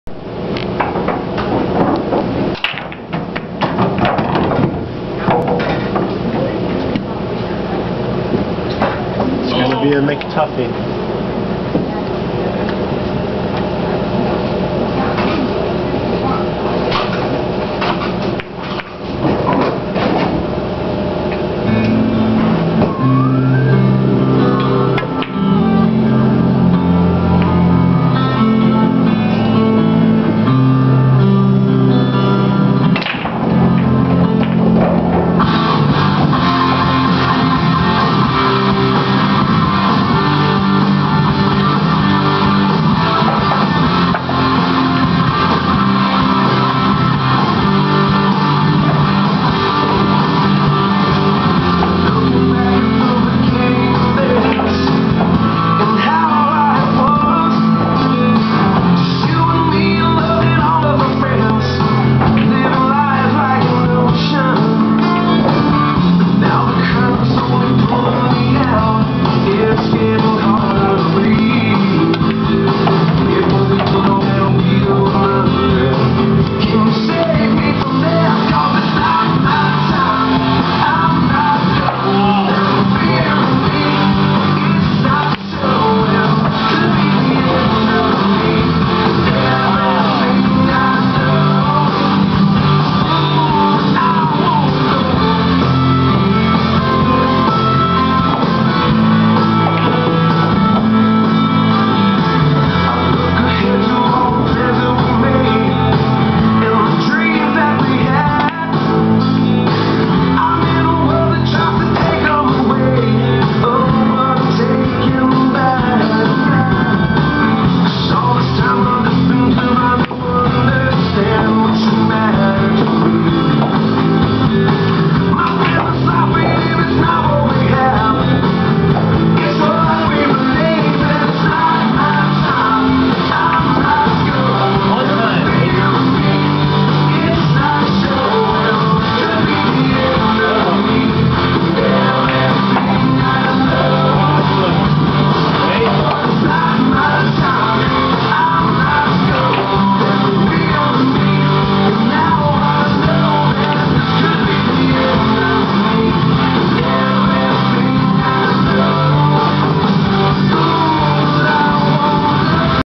Zach vs Gibbs Final Game
The last game of the two set match between Zach and Gibbs. Zach won the first set to give him a chance at winning the tournament.